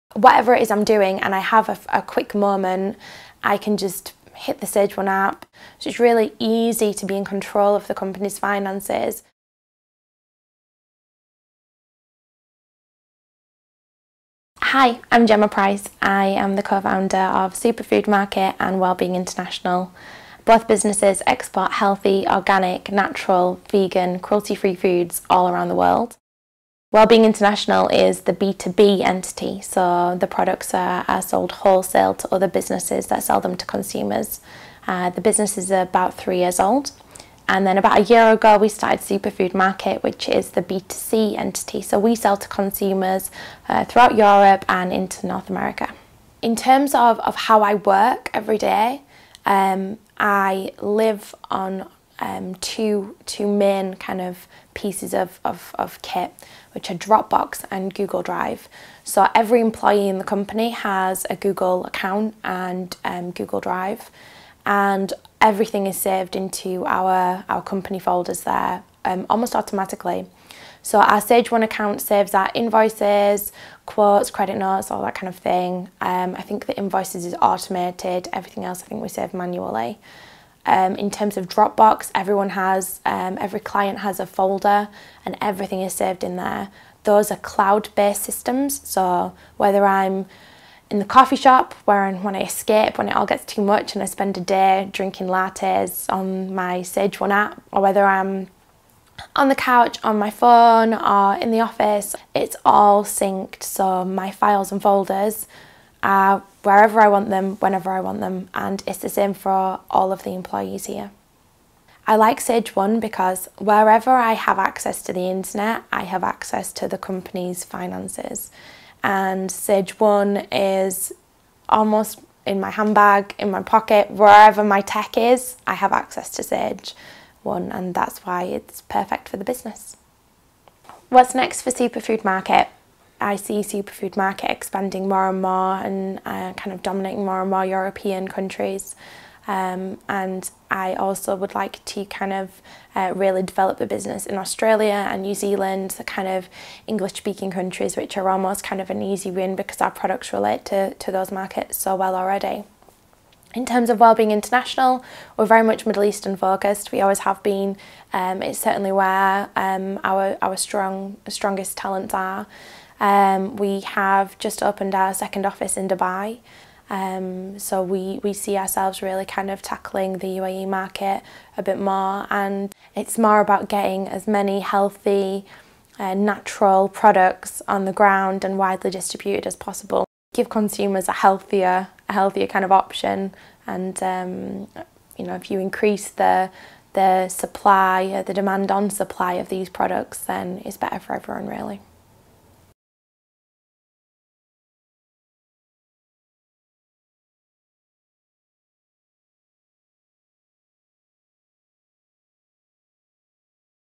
Gemma Price uses Sage Business Cloud Accounting
Gemma uses Sage Business Cloud Accounting, formerly known as Sage One to manage her business on her Mac, Macbook Air, iPad or iPhone and says "I like Sage Business Cloud Accounting because wherever I have access to the internet, I have access to the company's finances. Accounting is in my handbag, in my pocket, wherever my tech is - and that's why it's perfect for the business."

Find out more and take a free 30 day trial today